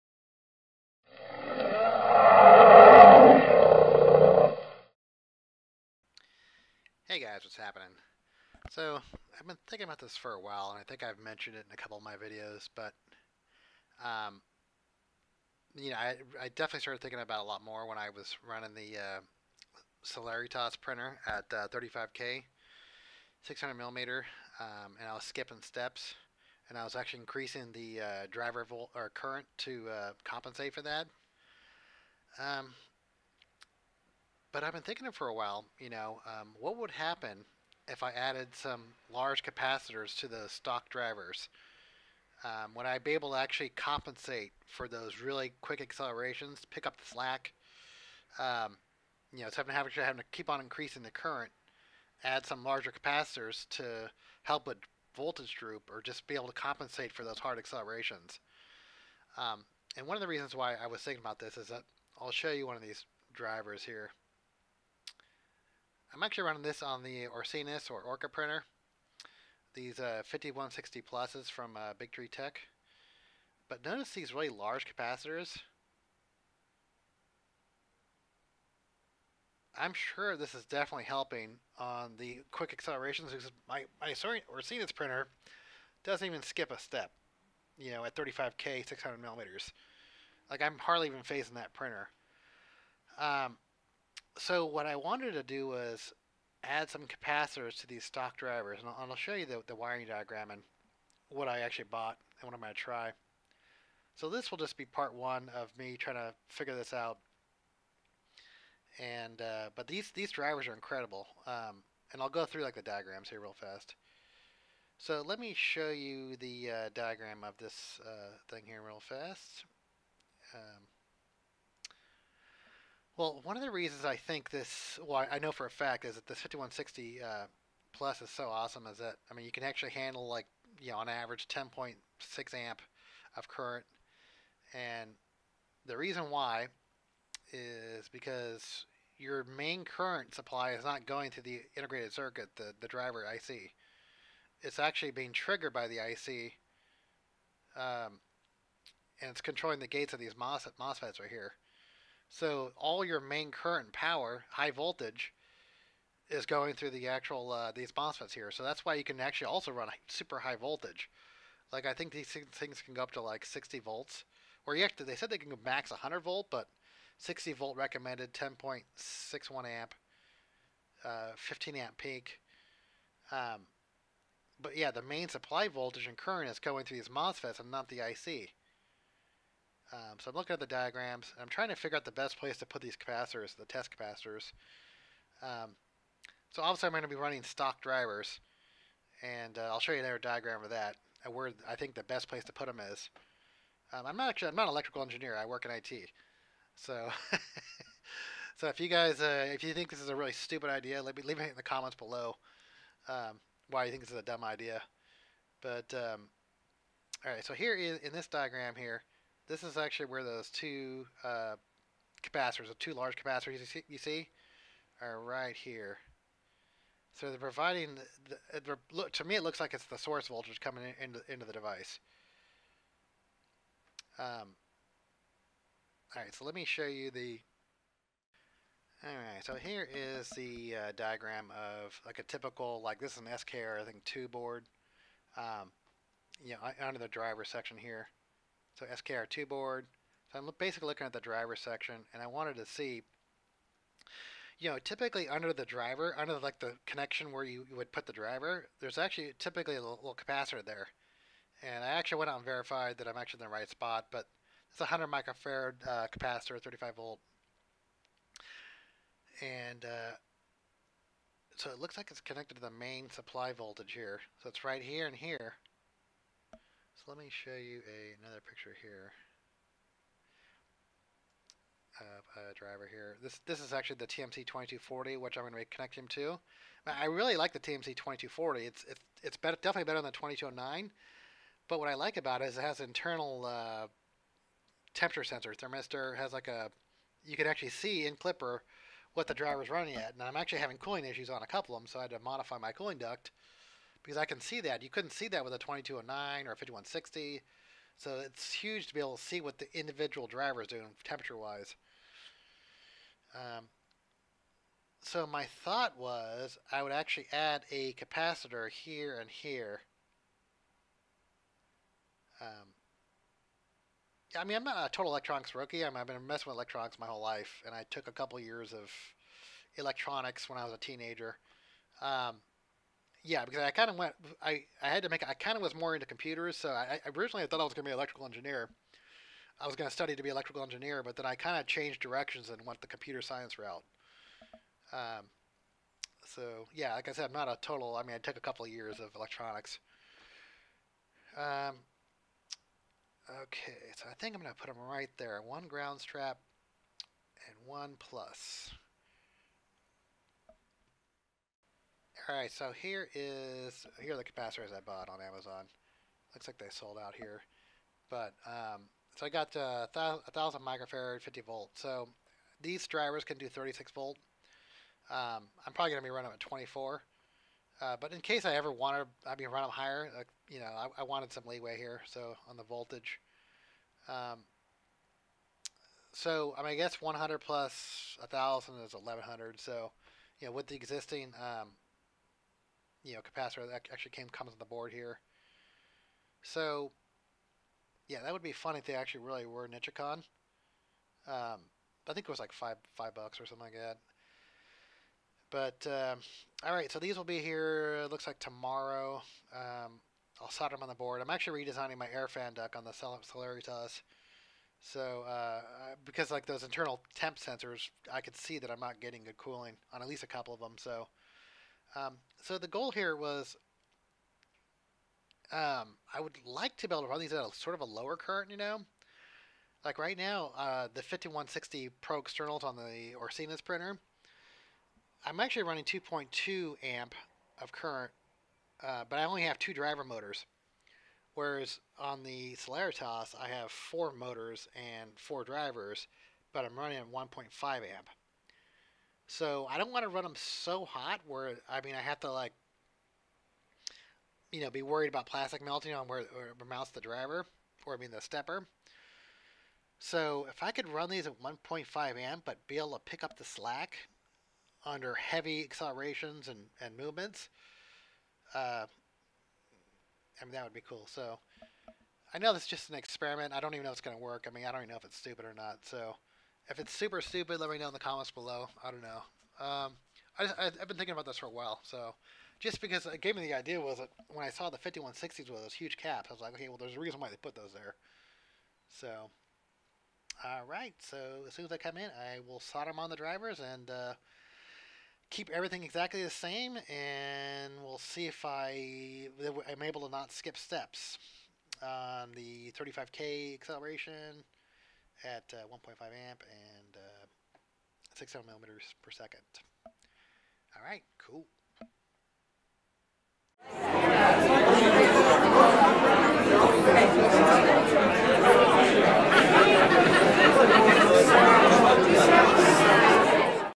Celeritas adding large capacitors to TMC2240 to prevent voltage current drop on hard accelerations 1
I've been thinking about this for awhile. What would happen if added some large capacitors on my BigTreeTech TMC2240 drivers. during hard accelerations my stepper motors skip steps at 1.5A. If I keep adding current to compensate my motors and drivers will heat up. My thought was to add some large capacitators to act like a buffer to pick up the slack. My BigTreeTech TMC5160 Plus external  don't even skip a beat at 600mm/s 35K acceleration.  Let me below if you think this is a dumb or good idea in the comments?

Capacitors I'm using

TMC5160 Plus

If you found this video helpful or would like help fund future experiments.